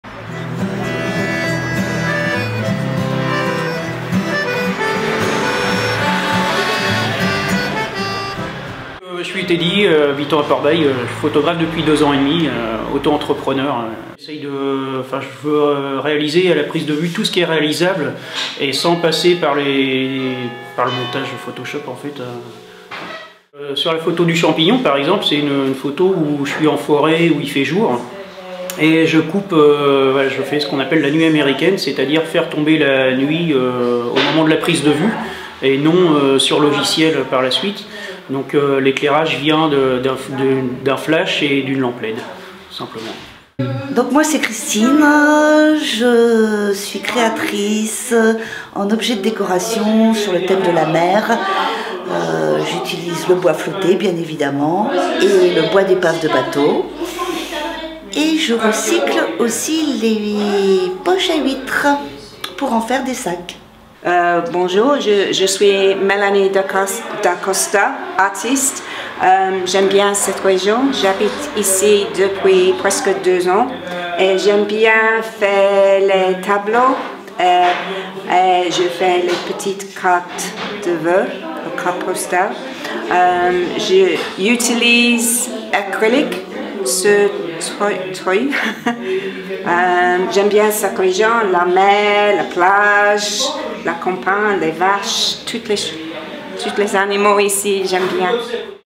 Portbail1
3 artistes de l'espace de la MMAAP (Maison des Métiers des Arts et Artisans ) de Portbail (Manche) vous présentent leur travail